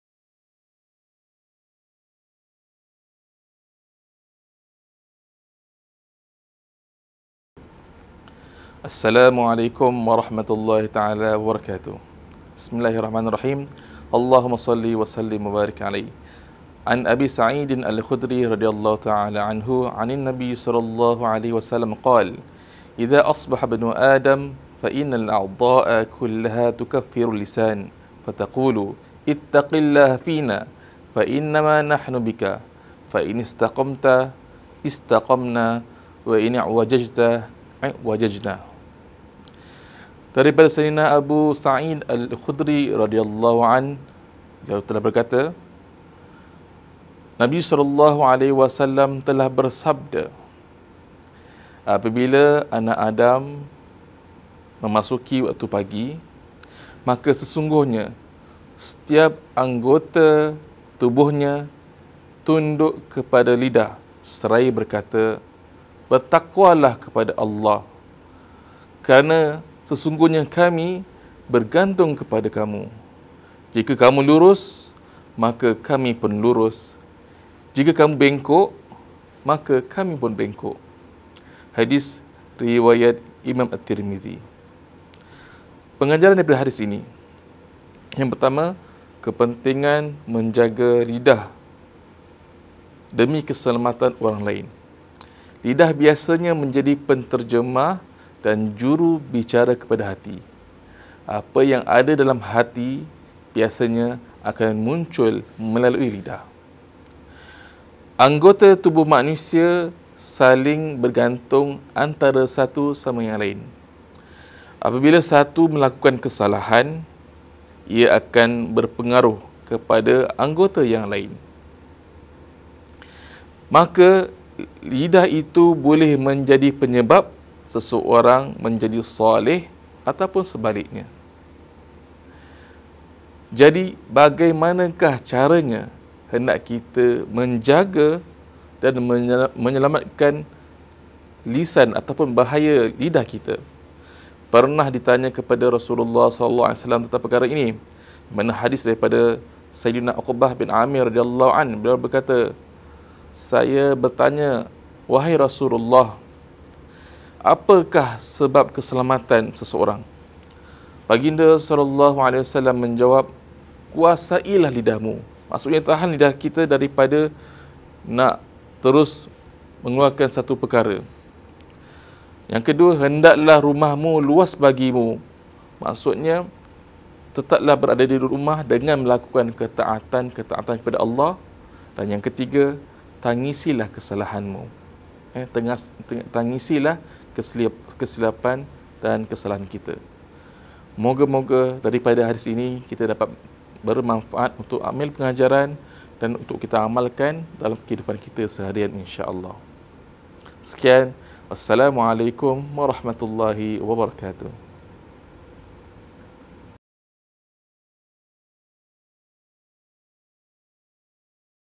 "Istiqamah Jaga Lidah"
Pengajaran Hadis Pilihan ﷺ | "Istiqamah Jaga Lidah"

Jadikan Al-Quran & Hadis sebagai panduan hidup kerana ia adalah cahaya yang dapat menerangi jalan yang gelap. Bersama kita menghayati Ramadan dengan penghargaan disebalik ujian.